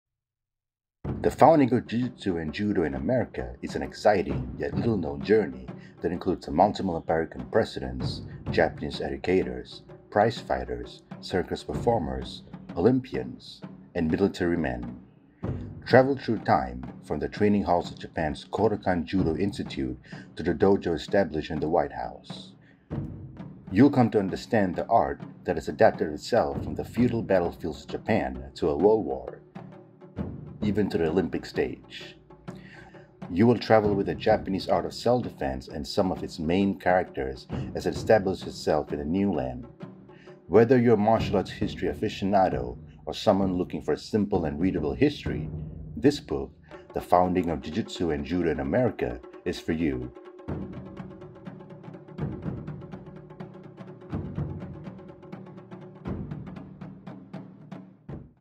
Founding of Jujutsu & Judo in America 🇺🇸 🥋
The Founding of Jujutsu & Judo in America is a book by George Rego with contributions by Abdul Rashid. This best selling book tells the fascinating story of how the Japanese art of self-defense journeyed from its homeland to the United States. In addition to outlining the story, characters, and events that laid the foundation of the expansion of the art in America, the second half of the book consists of five extensive interviews with pioneers (or senior students of those pioneers) of jujutsu and judo in the USA.

[Video by Abdul Rashid]